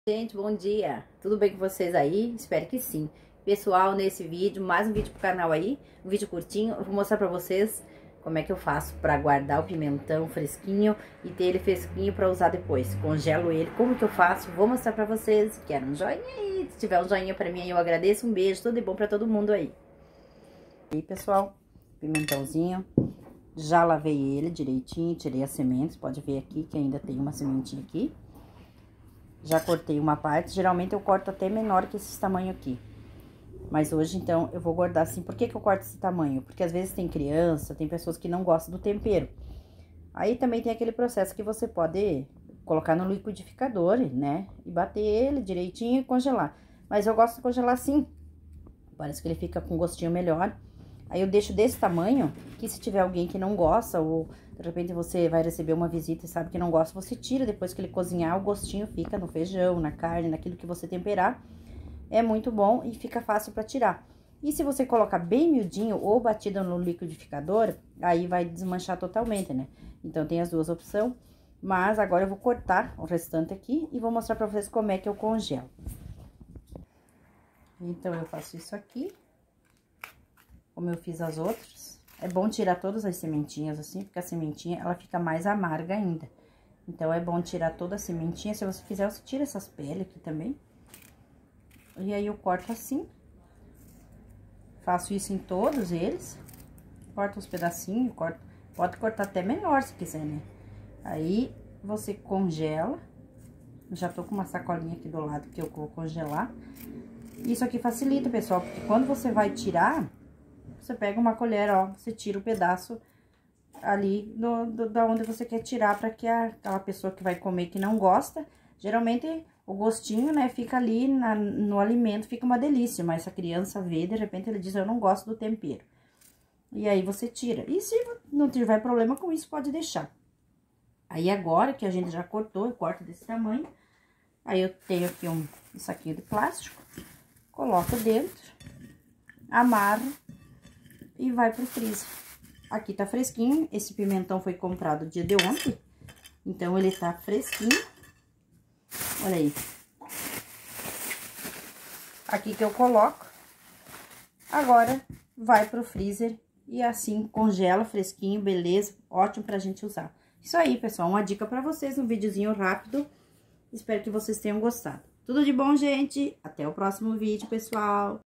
"Congelar Pimentão? Claro! Pra Usar Quando Quiser Veja como faço"
Neste vídeo, te convido pra aprender comigo se você gosta de pimentão e quer conservá-lo por mais tempo, dá pra congelar, claro que sim, é usar quando quizer,  veja como eu faço, ele conservar o sabor, seu alimento fica com o gostinho igual aquele pimentão comprado na hora na feira, vale muito a pena, e você economiza também, porque não tem risco de ficar na geladeira e depois de segunda fase estragar, uma ótima dica.

Quer me enviar um mimo?
Rua Argentina 122 Erechim RS
Ceep 99700026

Veja tbm os demais vídeos do canal.